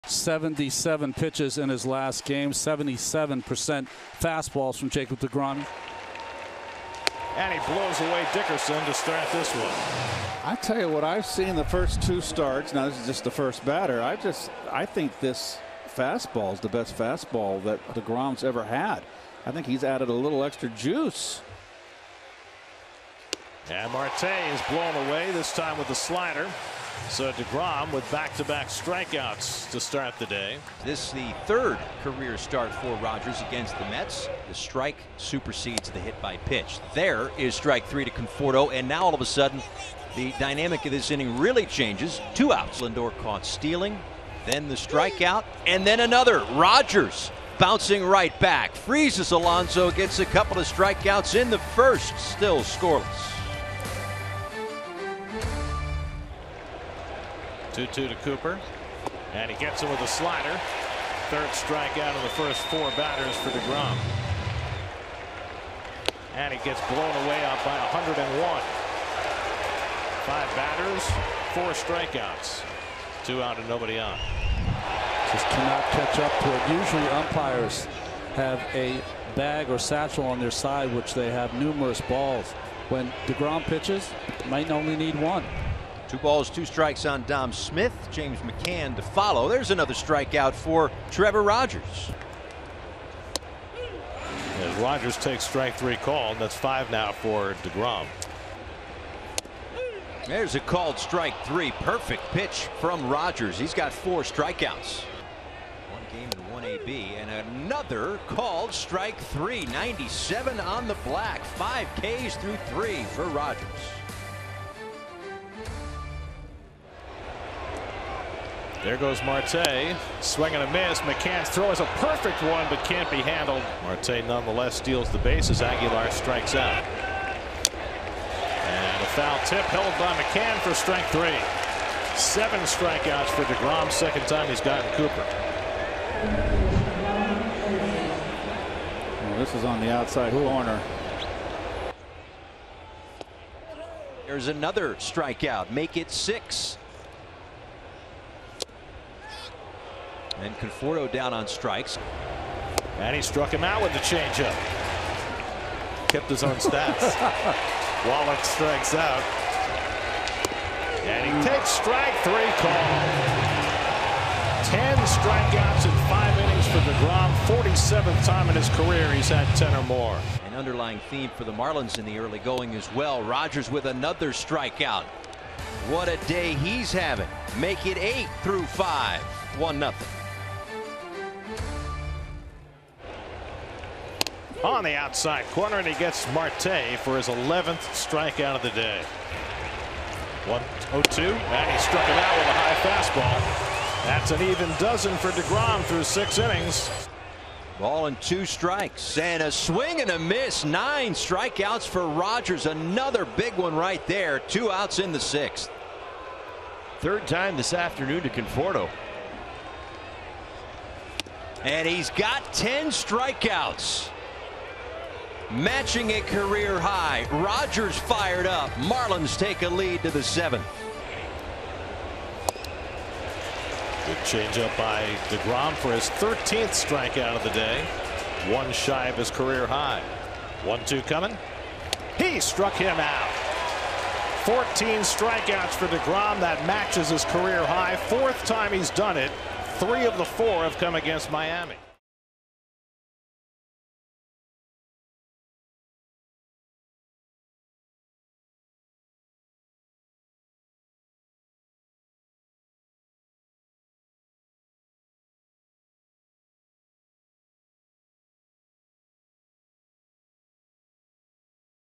Jacob deGrom and Trevor Rogers combine for 24 Ks in an old fashioned pitchers' duel!
deGrom and Rogers look in midseason form with their combined 24 K effort.